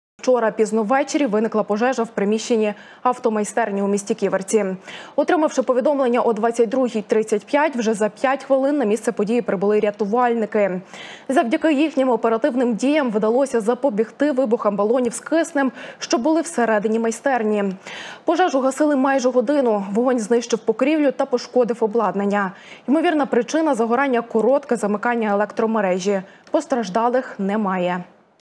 У Ківерцях пізно ввечері горіла автомайстерня
Горіла автомайстерня у місті Ківерці. Вчора пізно ввечері виникла пожежа в приміщенні автомайстерні у місті Ківерці.
Отримавши повідомлення о 22:35, вже за 5 хвилин на місце події прибули рятувальники. Завдяки їхнім оперативним діям вдалось запобігти вибухам балонів з киснем, що були всередині майстерні.
12 Канал у телеграмі, усі важливі новини Волині, України і світу.